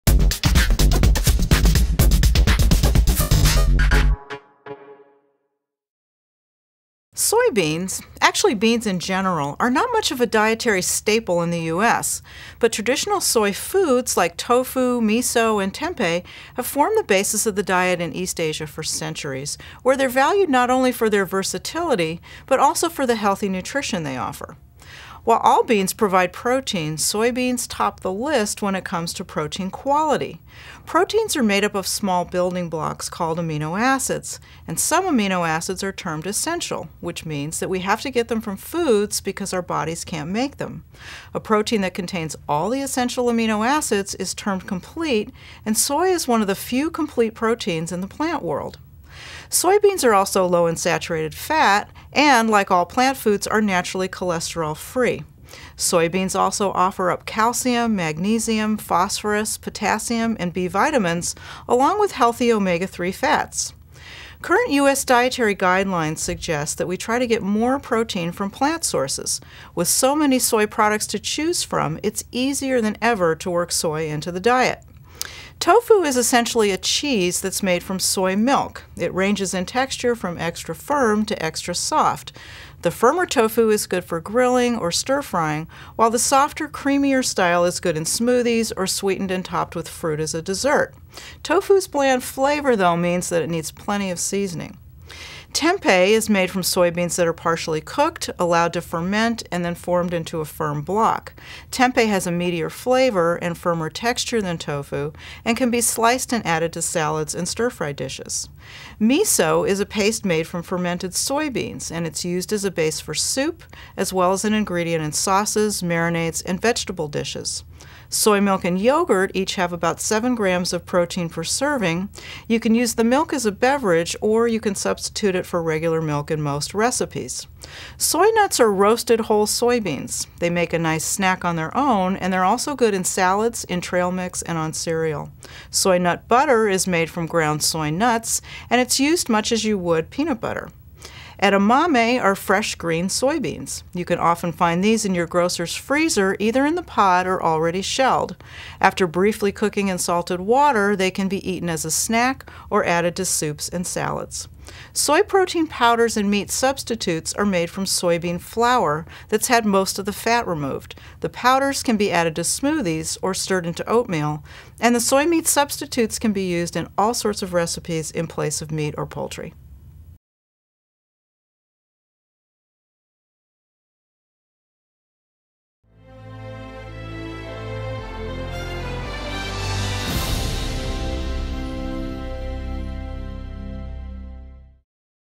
Soy and Soy Products -- Healthy Plant-Based Proteins
Beans in general are not much of a dietary staple in the US.  However, traditional soy foods like tofu, miso and tempeh, have formed the basis of the diet in East Asia for centuries, where they're valued not only for their versatility, but also for the healthy nutrition they offer.